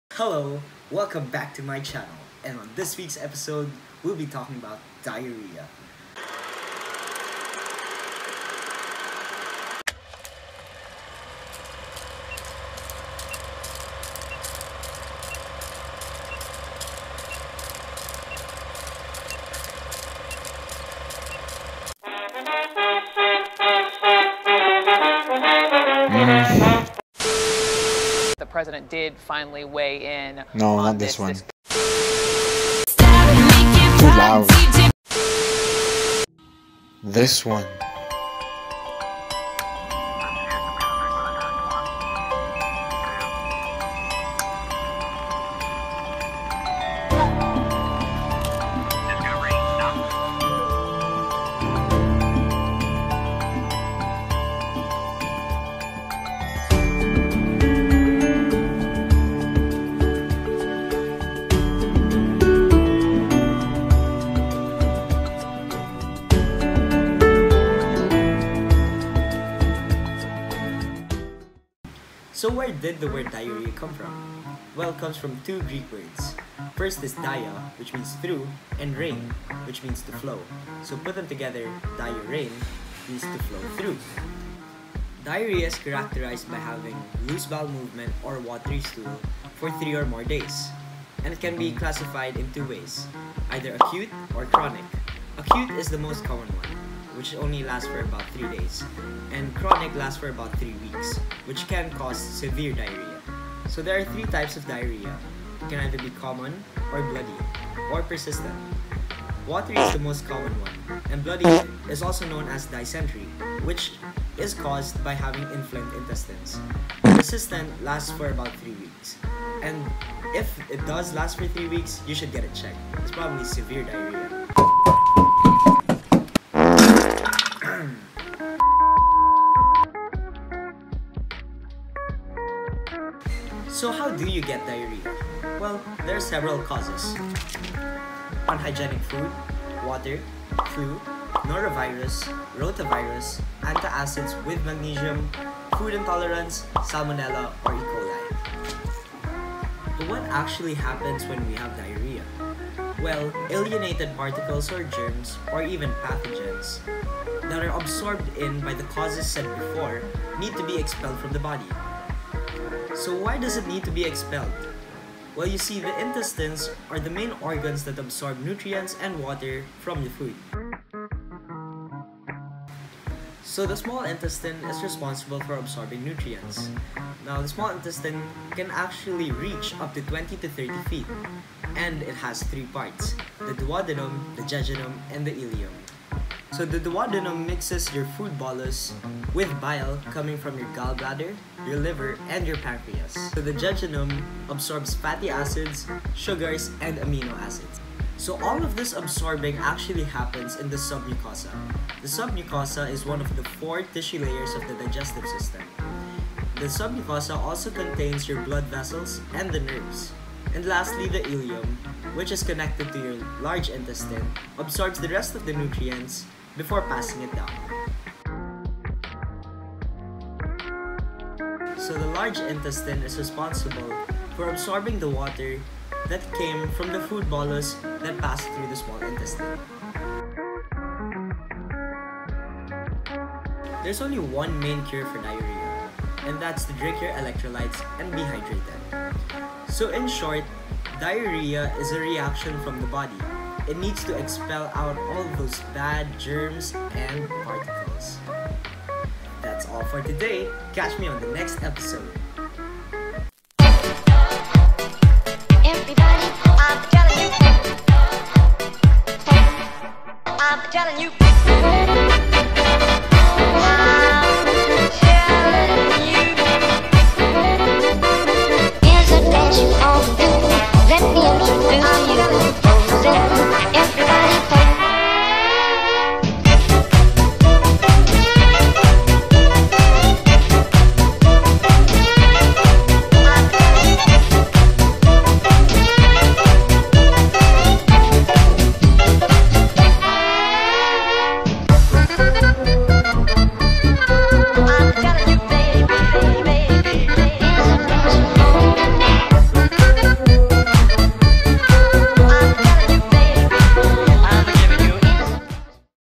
What is Diarreha?-The Beauty of Science
Episode 1: What is Diarreha?

An in depth video about Diarreha, its causes, and everything related to it!
Cast:
Dave Omila- Host
Justin Avendaño- VFX and Editor
Ryan Vicedo- Researcher and Scriptwriter

All the stock footage and music used in this video are for entertainment and educational use only.